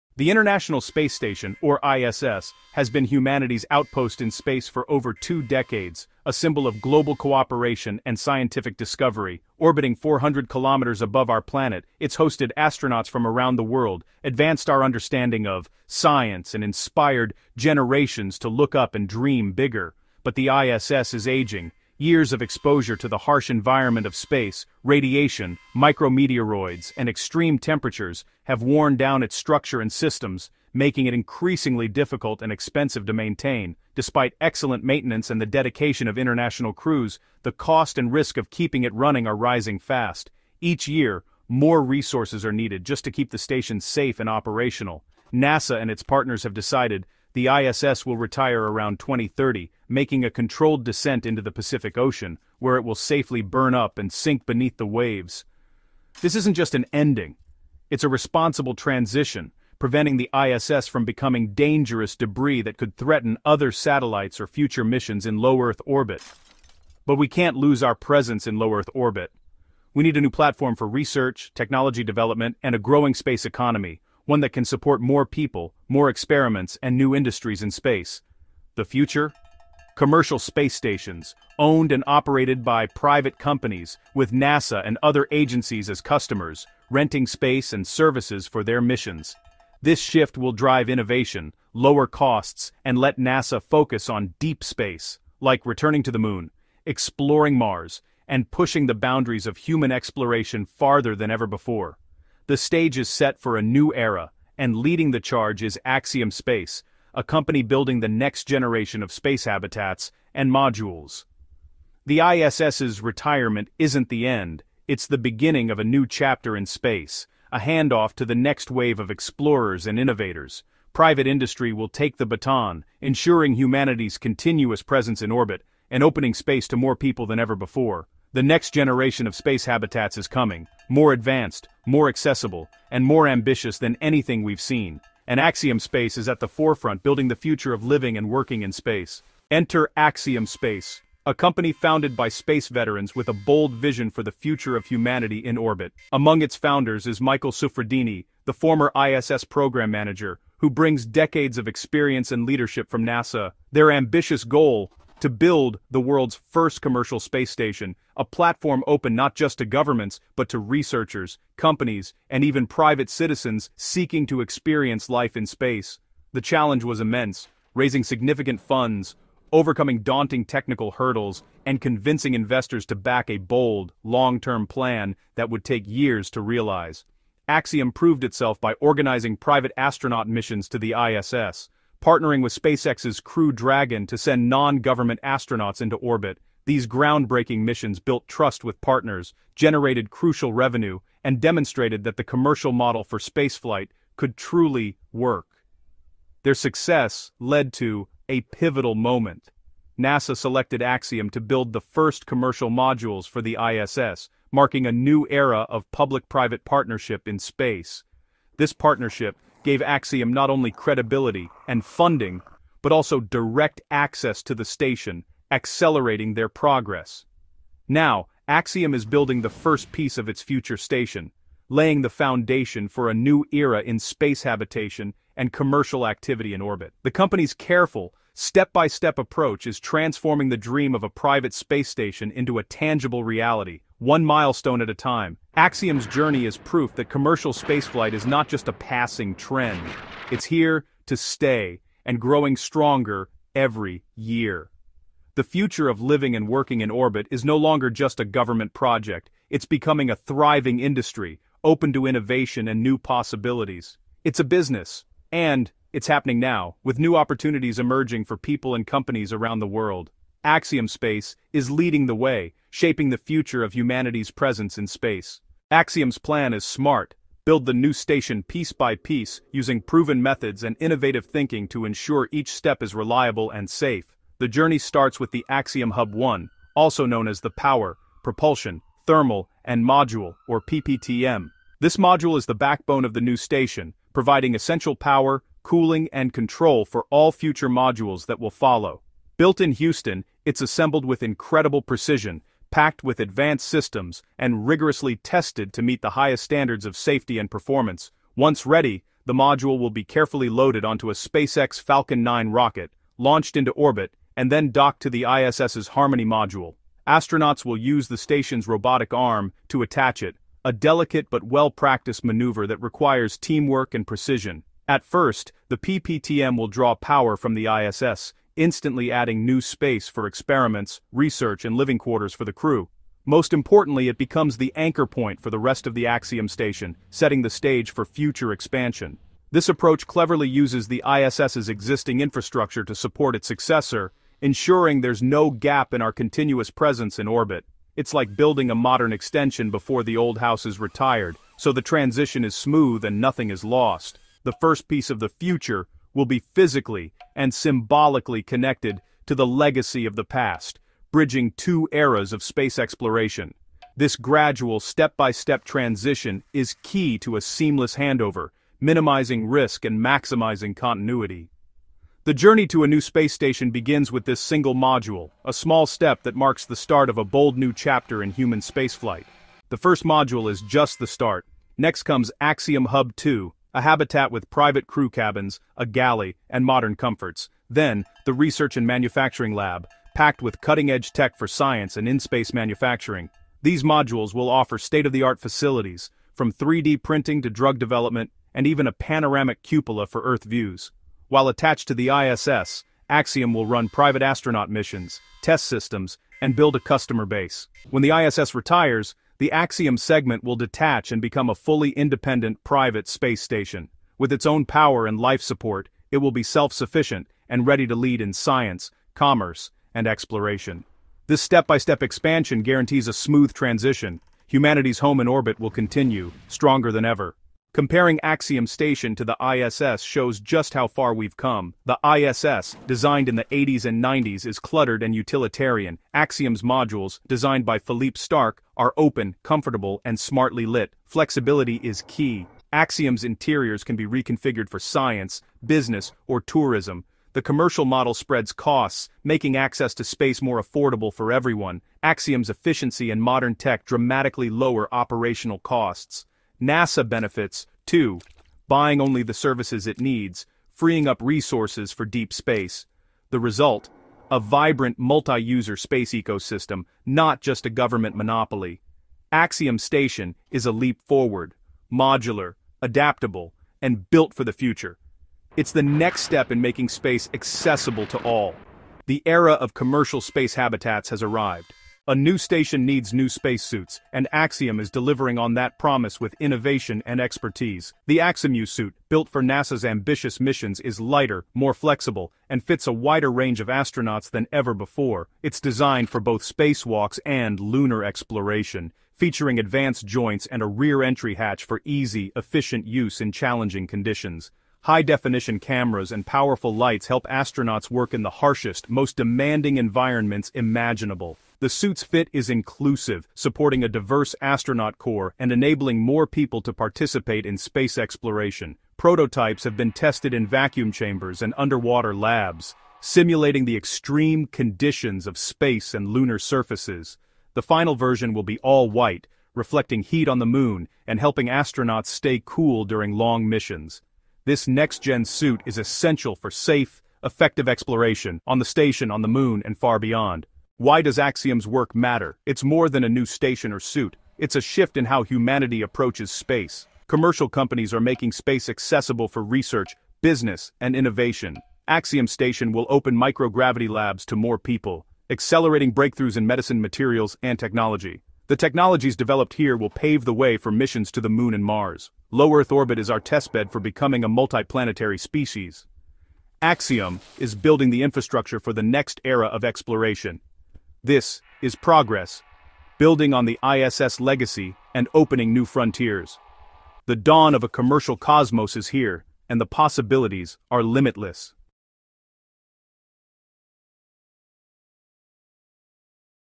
Inside SpaceX’s New Space Station: The Radical Replacement for NASA’s ISS Revealed!
SpaceX is quietly shaping the future of human spaceflight—and their new space station project is far more ambitious than anyone expected. As the ISS approaches retirement, SpaceX and its partners are preparing a next-generation orbital outpost designed for private missions, commercial research, space tourism, and long-duration exploration. In this video, we break down how SpaceX plans to replace the ISS, the technology behind their upcoming modules, why NASA is depending on them, and how Starship could unlock an entirely new era of life in orbit. From modular habitats to Starship-launched mega-structures, this is everything you need to know about SpaceX’s future space station and why it will change low-Earth orbit forever.